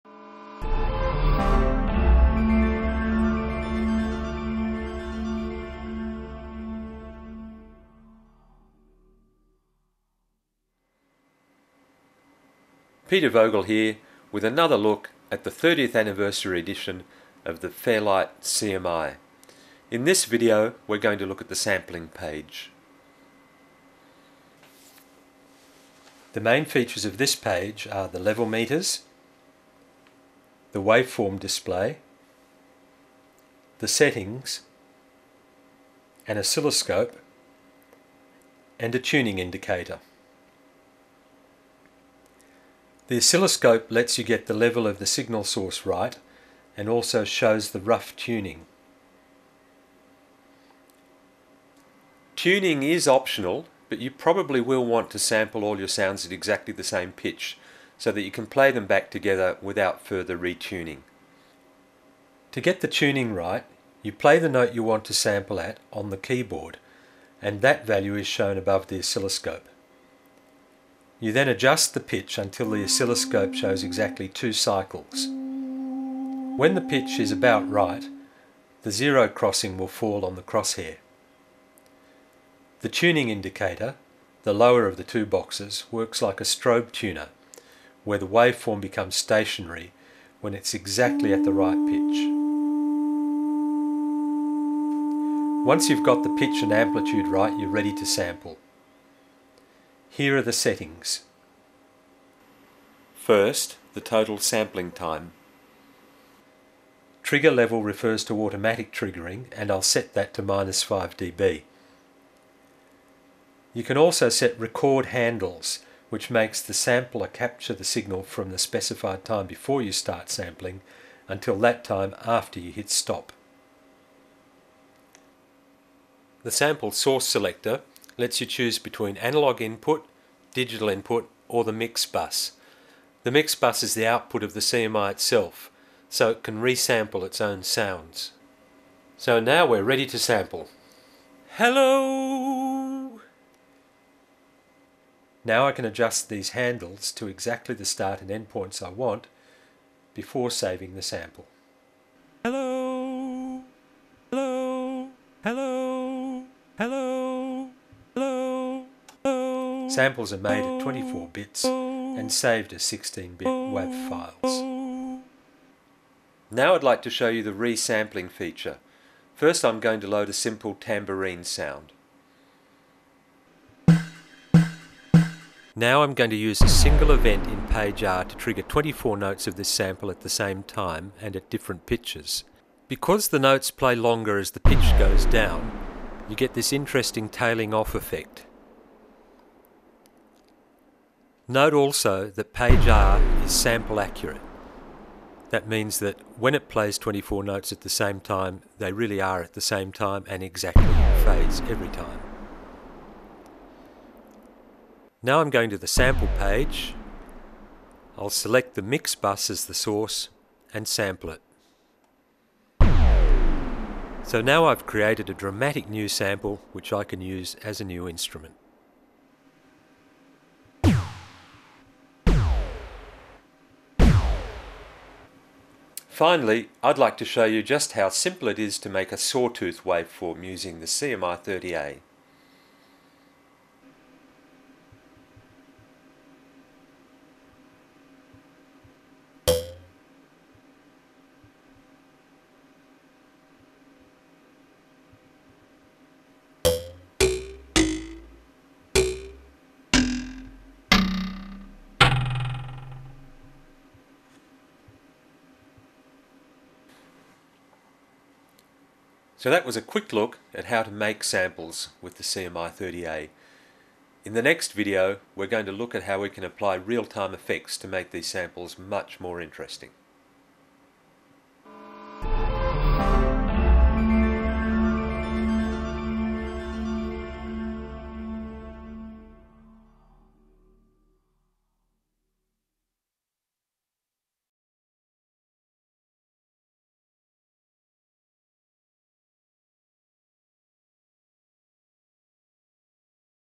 CMI30A-sampling
Peter Vogel demonstrates sampling using the CMI-30A, a thirtieth anniversary remake of the iconic Fairlight CMI of the 80s, now brought back to life by Peter Vogel Instruments.